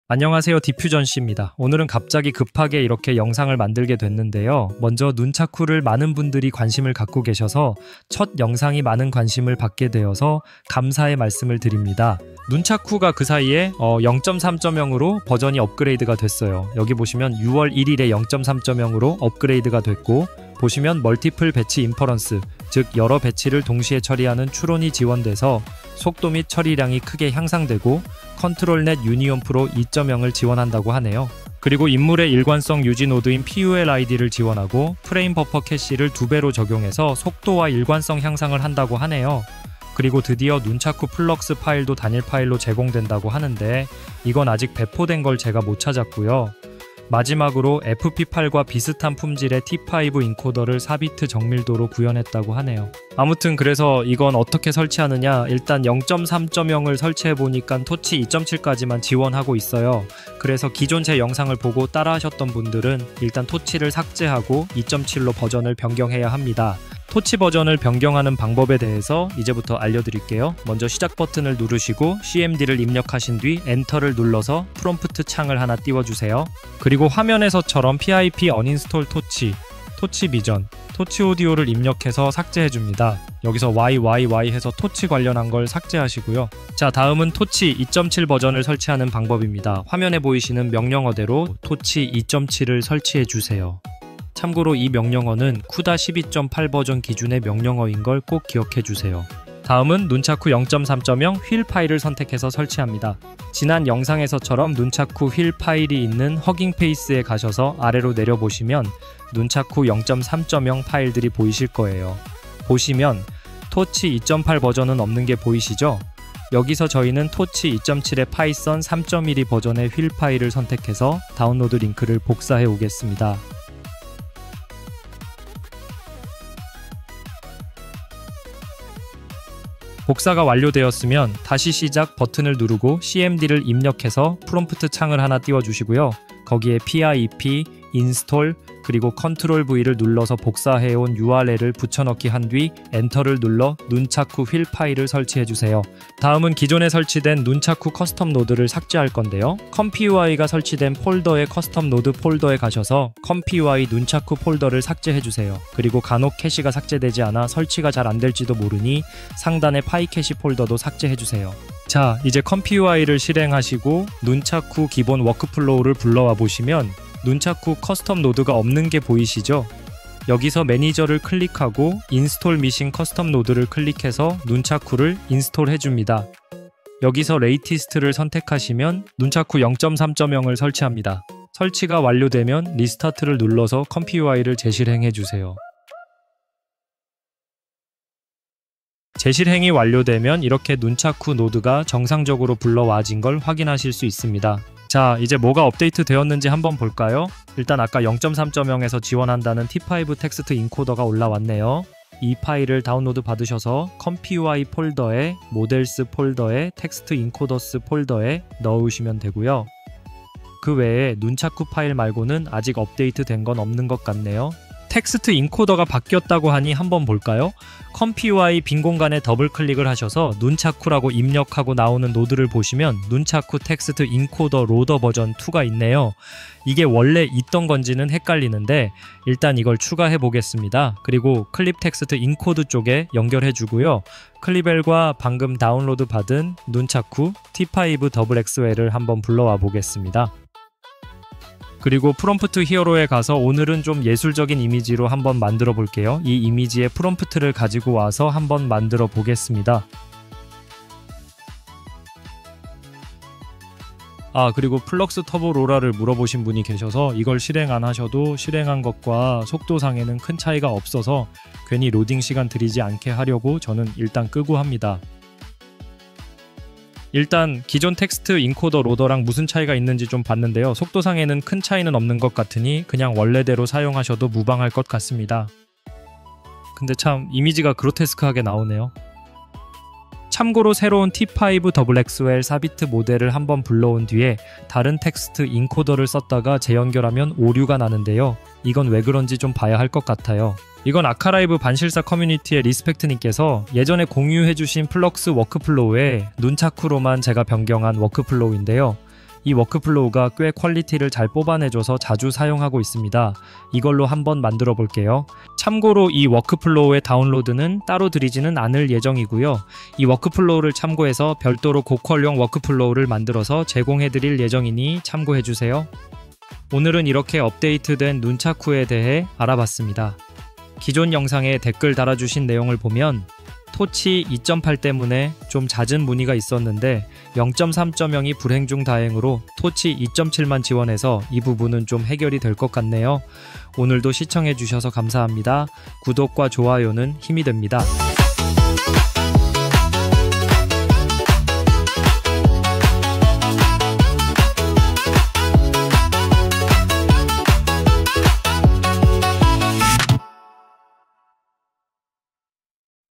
Nunchaku 0.3.0 업데이트! 설치방법을 공개합니다!
👉 Nunchaku가 지난 6월 1일, 0.3.0으로 업데이트 되었습니다. 기존 0.2.0 사용자 분들은 torch를 2.7로 재설치하고, 0.3.0으로 업데이트 해보세요. 드디어 torch 2.8 dev 버전의 두려움에서 벗어날 수 있습니다!!!
(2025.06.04 한국시간 오후 5시 30분 기준) 2시간 전, torch 2.8 지원 버전의 nunchaku 0.3.0 whl도 업데이트 되었습니다.
0.3.0 업데이트 이후 더 이상 폴더채로 모델을 가져가지 않고, 단일 파일로 제공됩니다.
각각의 파일을 다운로드 후 Models/diffusion_models에 넣어주시면 됩니다.

일반적으로는 int4라고 기재된 파일을 받으시고, 블랙웰 아키텍처(RTX 5xxx)는 fp4라고 기재된 파일을 받으시면 됩니다.

🚀 Release v0.3.0!
이번 업데이트에서는 다음과 같은 주요 기능이 추가되었습니다:
- Multiple-batch inference 지원: 이제 여러 입력 배치를 동시에 처리할 수 있어, 인퍼런스 효율이 크게 향상됩니다.
- ControlNet-Union-Pro 2.0 통합: 더 정교한 컨트롤이 가능해졌습니다.
- PuLID 초기 통합: 인물 정체성 보존 및 맞춤화 기능이 향상되었습니다.
- Double FB Cache 도입: 프레임 버퍼 캐시를 이중으로 최적화해 반복 생성 시 성능과 일관성이 개선됩니다.
- Nunchaku FLUX models를 이제 단일 파일로 로드할 수 있어 설치와 사용이 더욱 간편해졌습니다.
- 업그레이드된 4-bit T5 encoder는 이제 FP8 T5와 동등한 품질을 제공합니다.
[영상에 사용된 명령어 및 설치방법 링크]

00:00 인트로
00:16 Nunchaku 0.3.0 업데이트 내용
01:07 torch 2.7 버전으로 변경
01:39 nunchaku 0.3.0 whl 파일 설치
02:26 nunchaku 커스텀노드 재설치
03:17 4bit T5 xxl Encoder 설치
03:34 워크플로우 실행 및 테스트
04:51 '리스펙트'님 워크플로우로 테스트
05:16 맺음말